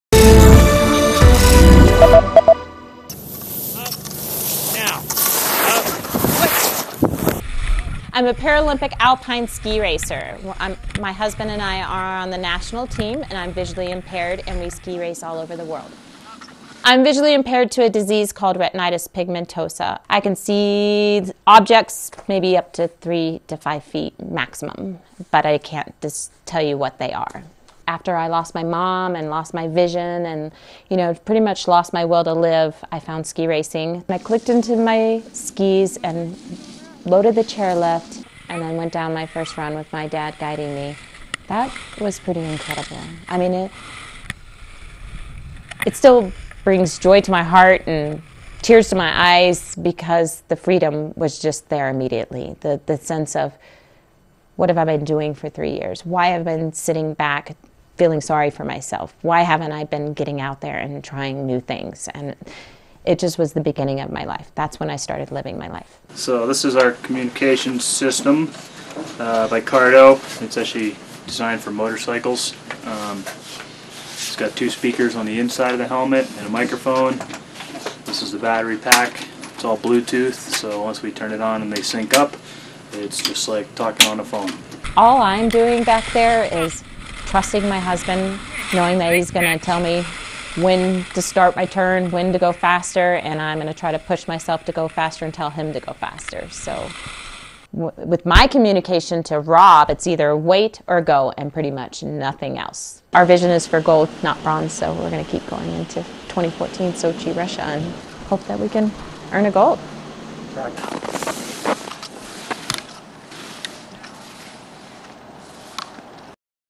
First Person: Blind skier's quest for gold
U.S. Paralympic bronze medalist Danelle Umstead is visually impaired, but she has a 'vision' for gold at the 2014 Paralympic Games. Umstead and her husband communicate through a special helmet that enables them to race down the slopes together. (Jan. 31)